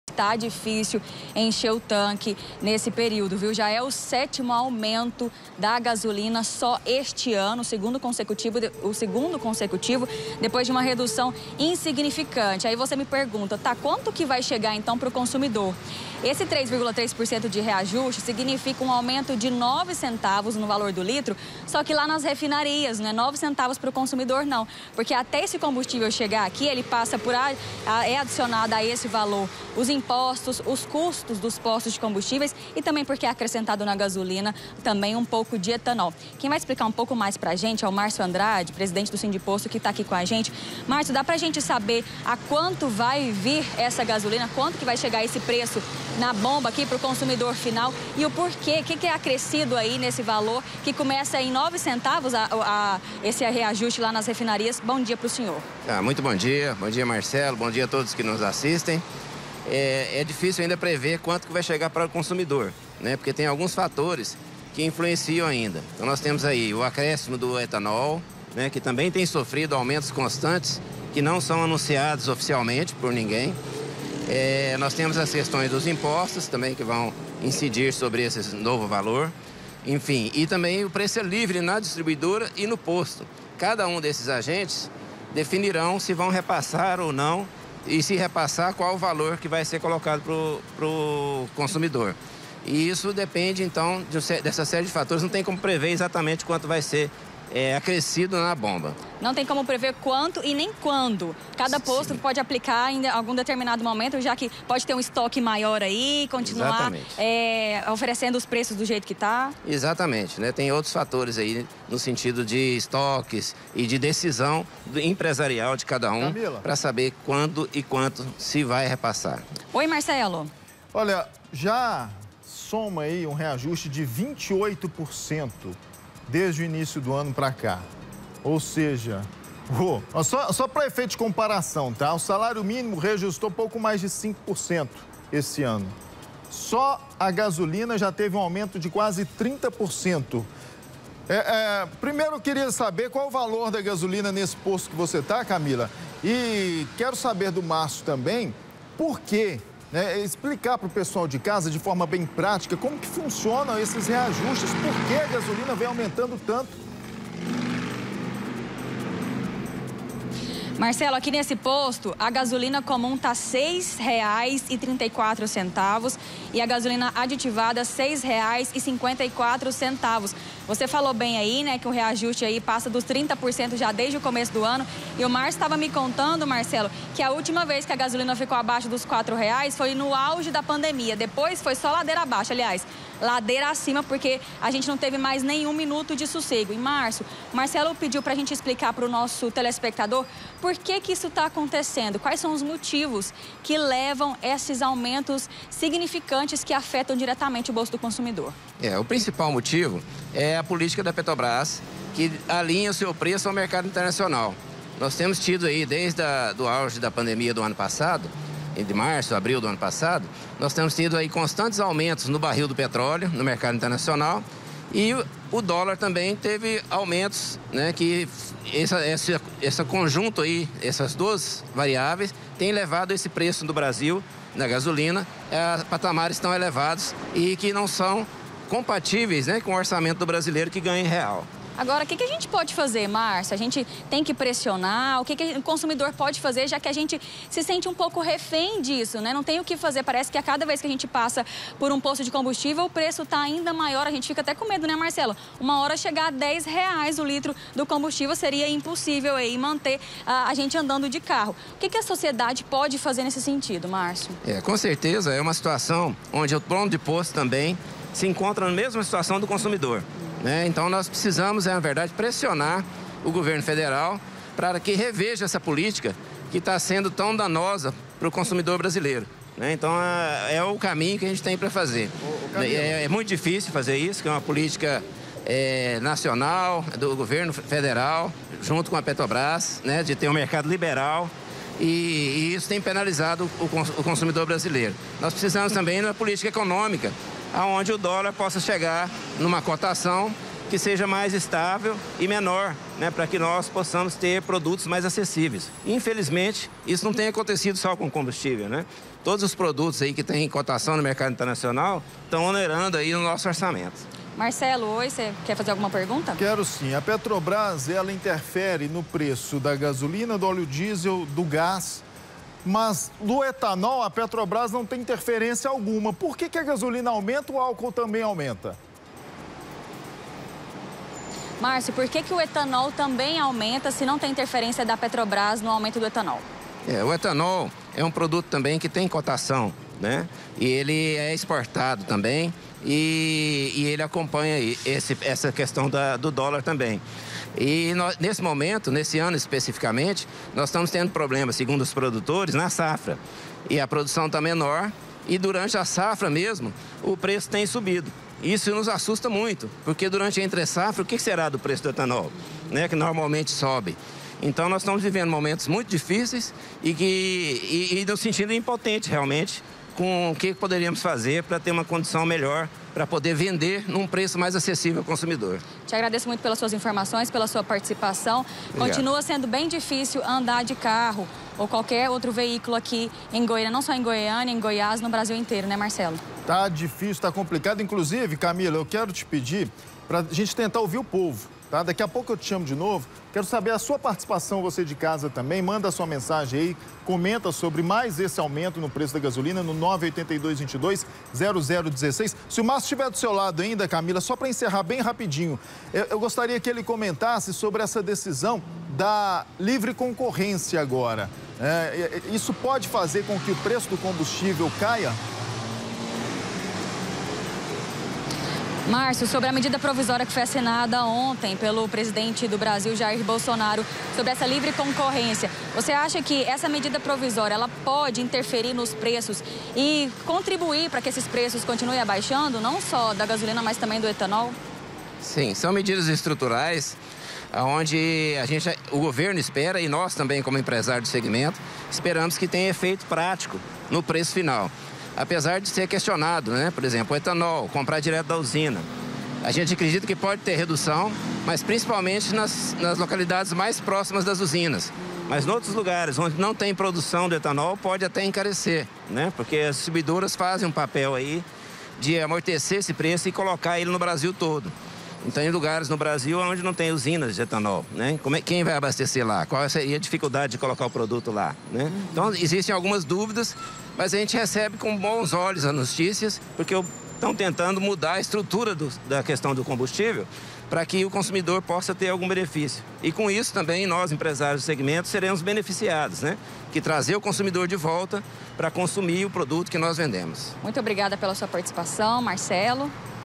MUITO CARO! HOJE TEM MAIS AUMENTO NO PREÇO DO COMBUSTÍVEL
A PETROBRAS ANUNCIOU MAIS UM AUMENTO NO PREÇO DO COMBUSTÍVEL NAS REFINARIAS DESTA VEZ DE 3,3% GOIÂNIA É HOJE A SEGUNDA CAPITAL COM A GASOLINA MAIS CARA DO BRASIL. SÓ PERDE PRO RIO DE JANEIRO.